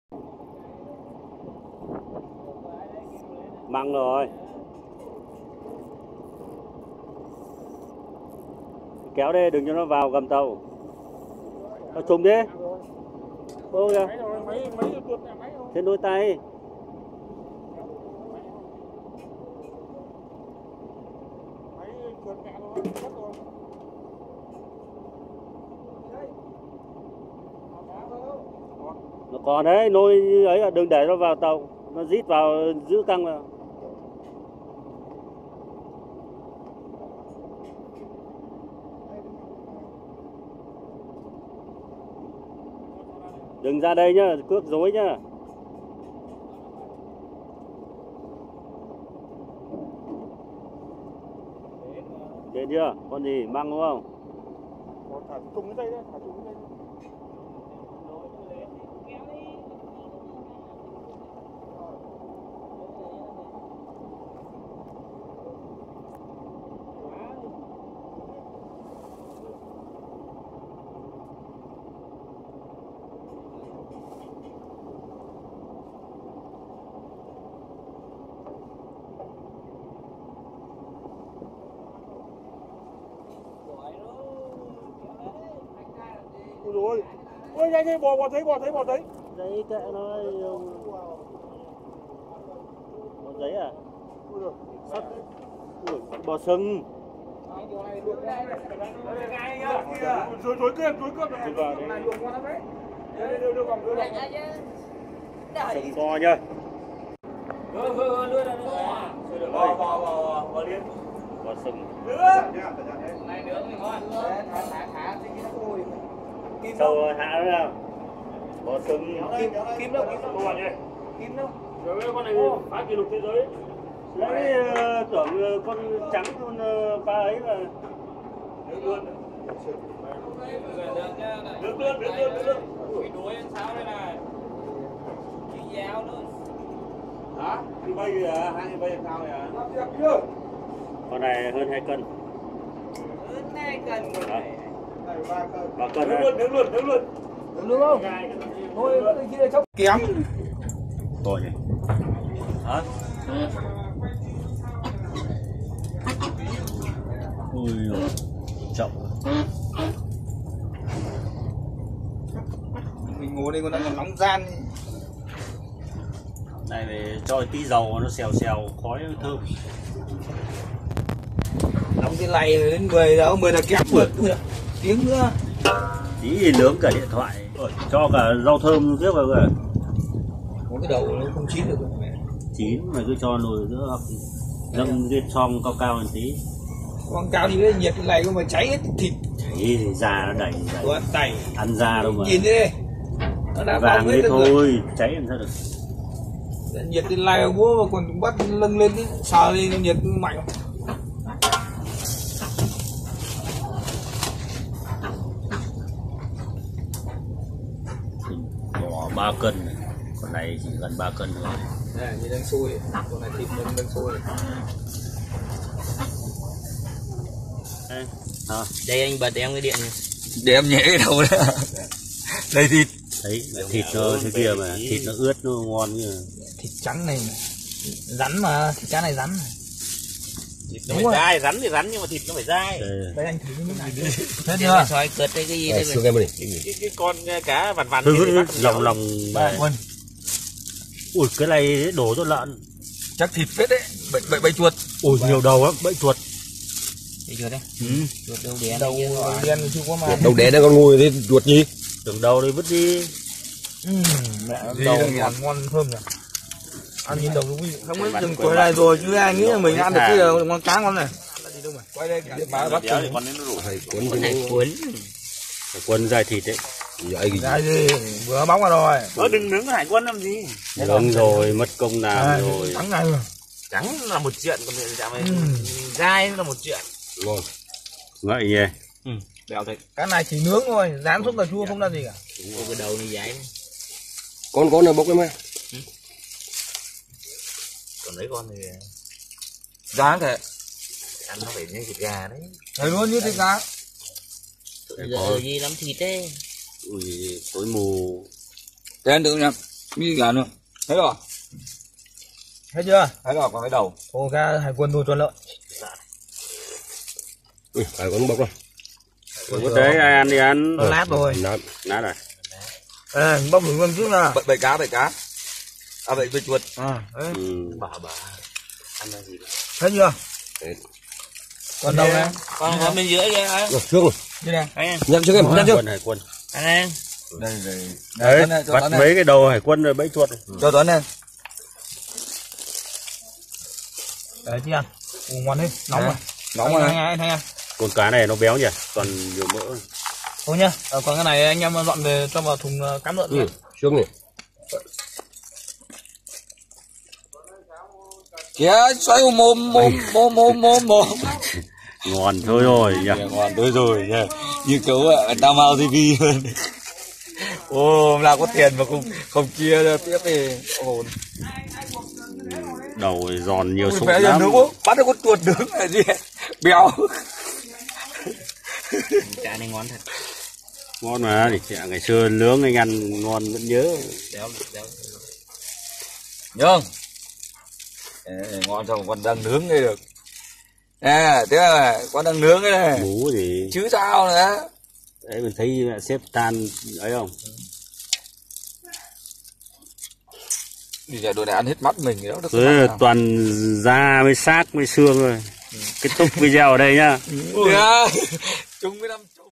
Câu lên. Nướng toàn cá Bò bọc thép, Bò sừng..
Câu Cá Sừng Nhọn Rất Nguy Hiểm. Trải nghiệm câu cá ngoài khơi xa. mùa dịch